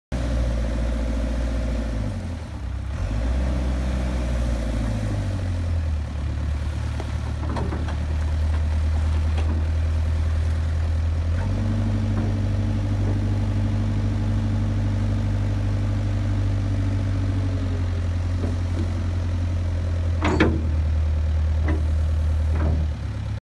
JCB 408B ZX ARTICULATED FARM LOADING SHOVEL
JCB 408B ZX Articulated Farm Loading Shovel c/w Forks, Bucket, Hydraulic Quick Release. For Auction Saturday 30th July 2016.View Live On-Line Bidding Available.